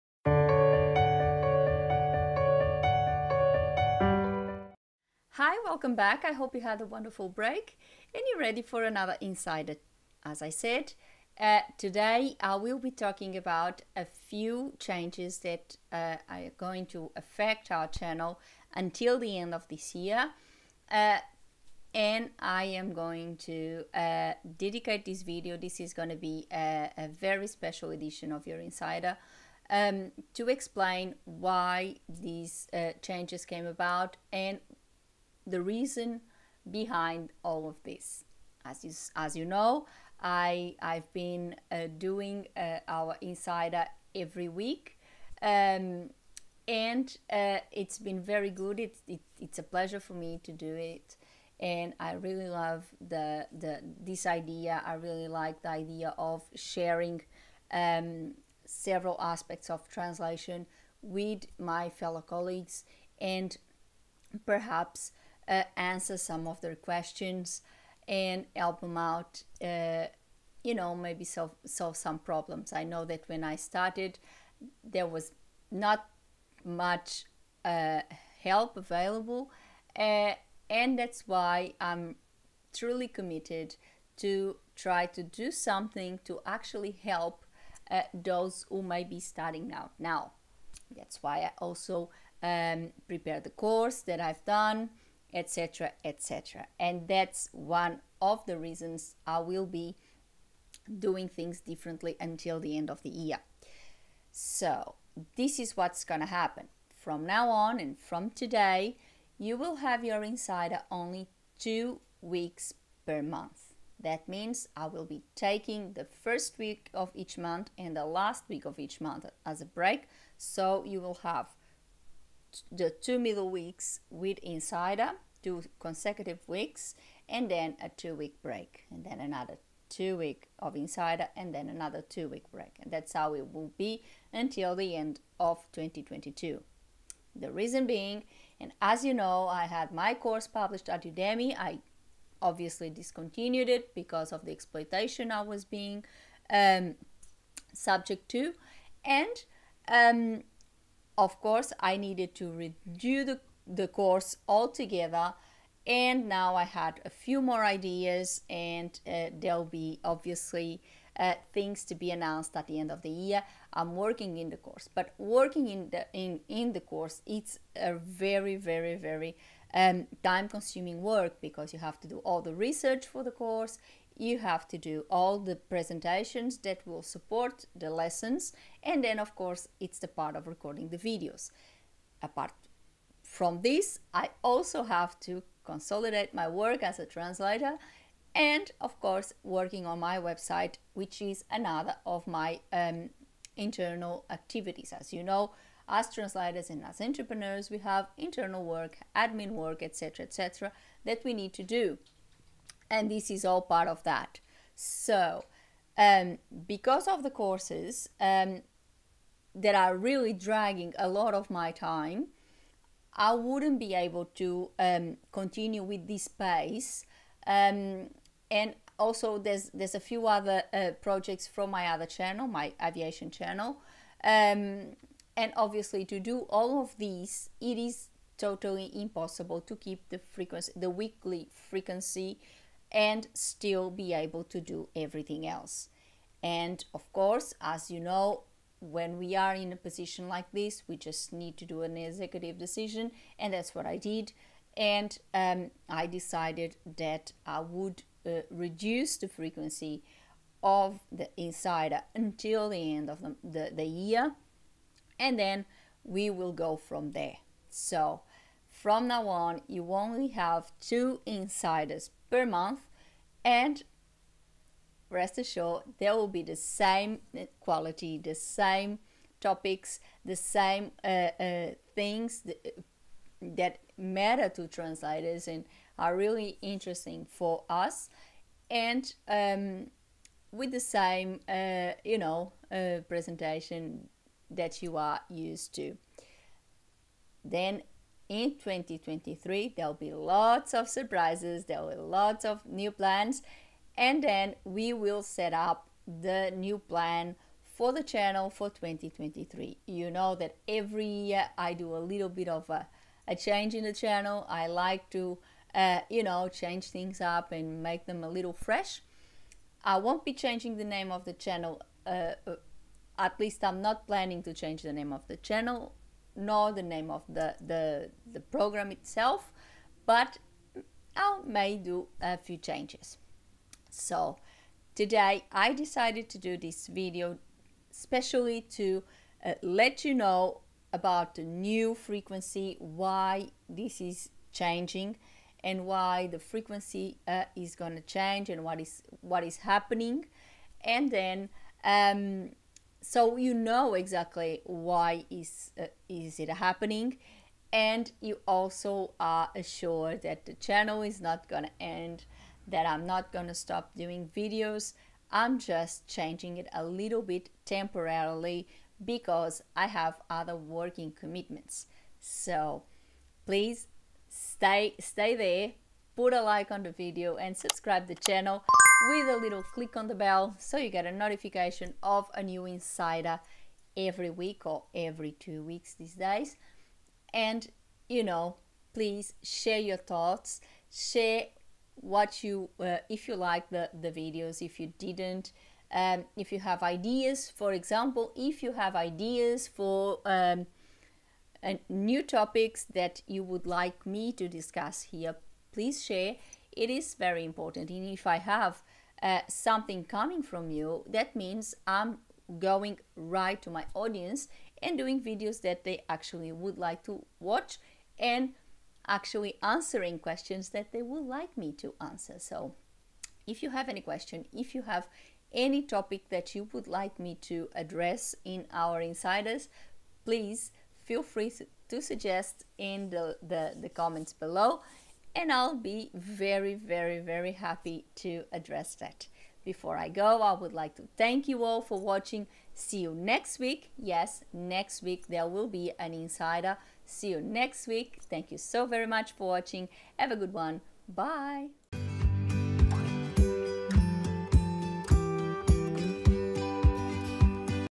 Temporary changes to the channel
Due to work commitments, the channel will have some temporary changes. This video explains all of those changes and the plans ahead.